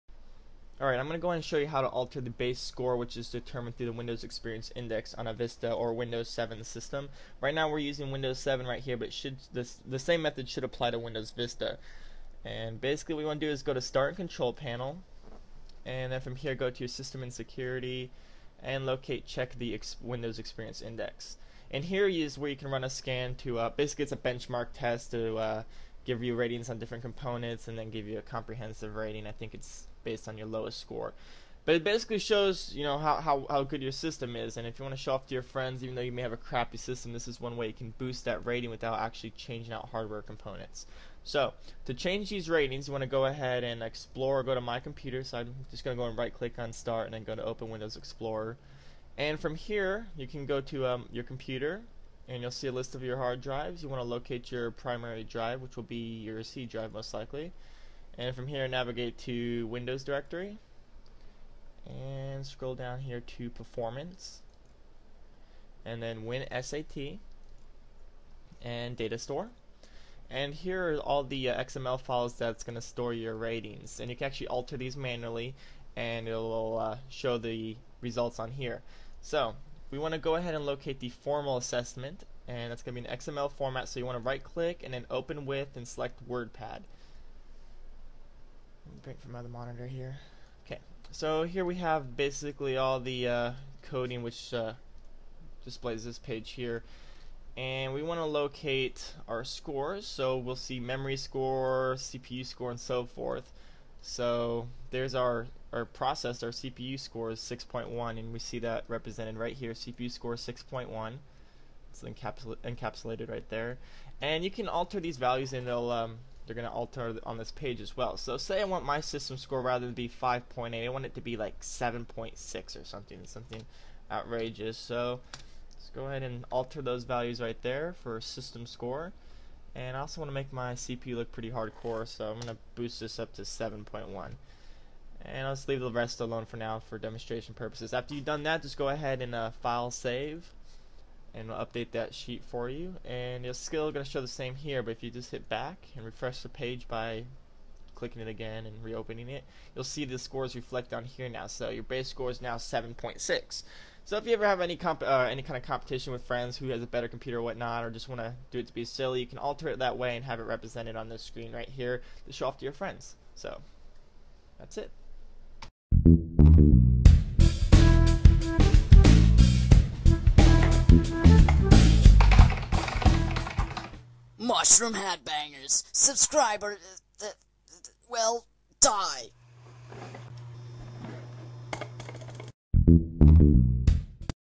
Manually Alter your Vista/Windows 7 Score
A tutorial on how to manually alter your systems score for the Windows Experience Index BenchMark on a Vista or Windows 7 machine.

NOTE - If you try to raise the Score above 7.9 it won't be displayed